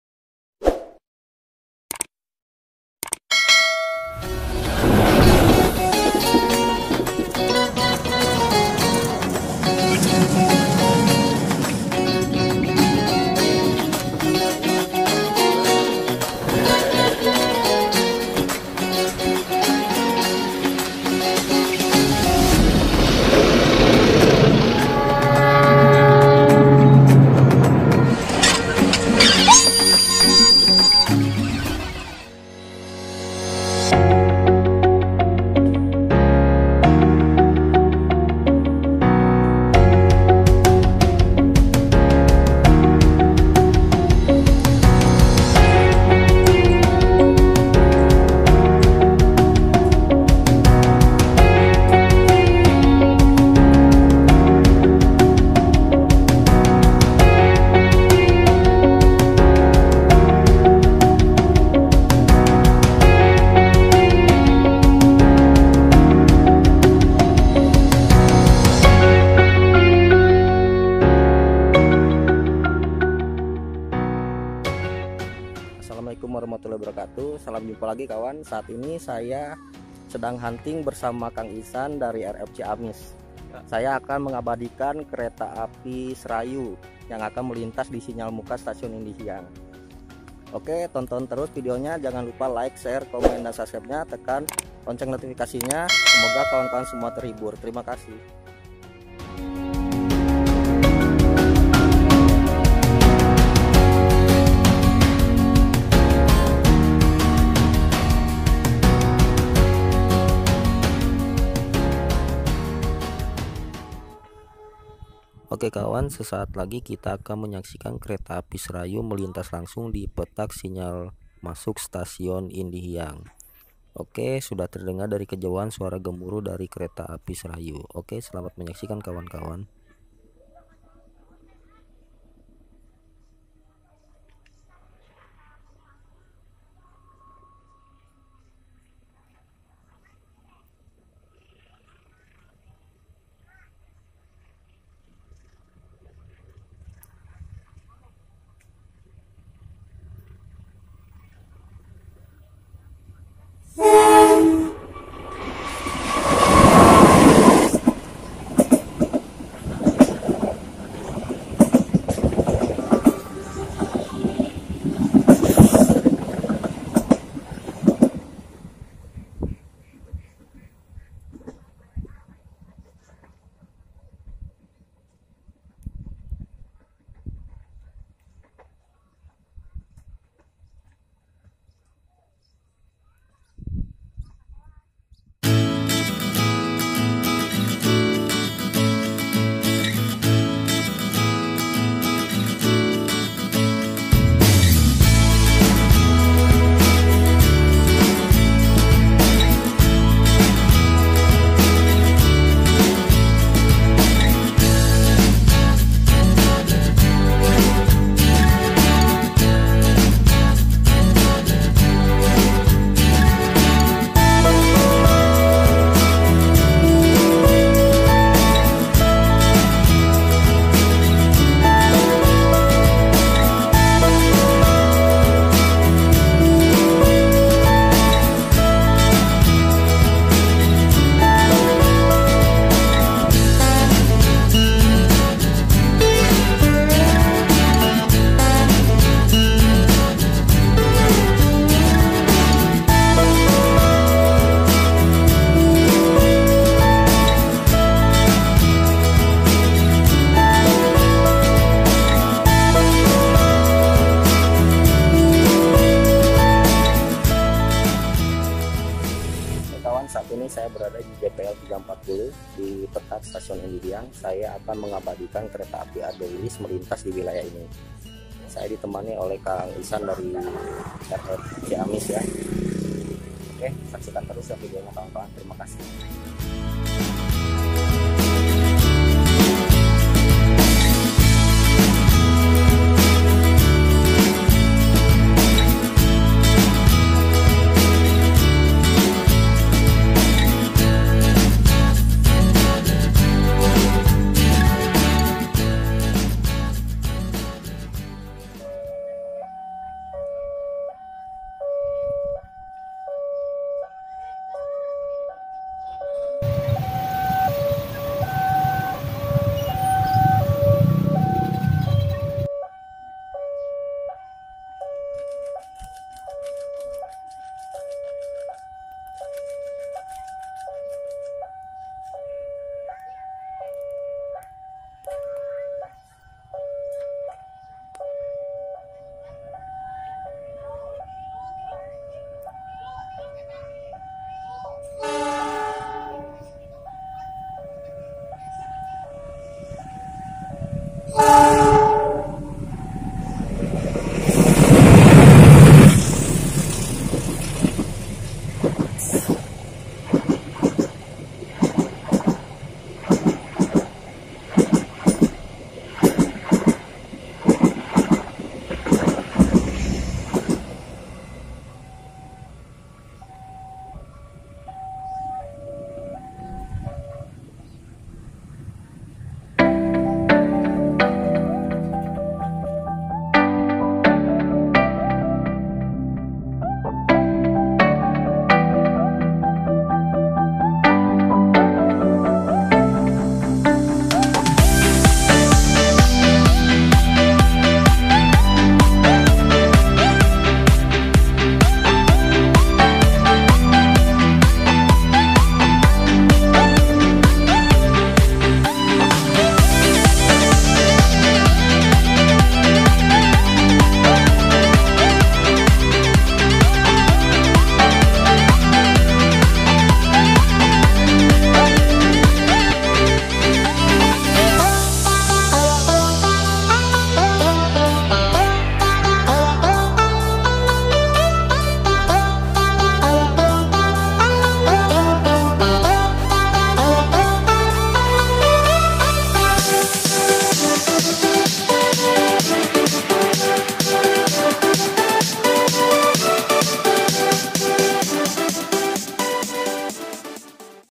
Hunting Kereta Api di Spot dan JPL 340 Indihiang Bersama Kang Insan | PART 1
Video Kali ini Saya Hunting Kereta Api di Spot dan Jpl 340 Indihiang bersama Kang Insan Part 1 dan kami mendapatkan moment kereta api melintas dan sapaan masinis juga semboyan 35. ok simak video kami sampai selesai Terima Kasih.
Lokasi JPL 340 berada di daerah Sirnagalih Indihiang Tasikmalaya.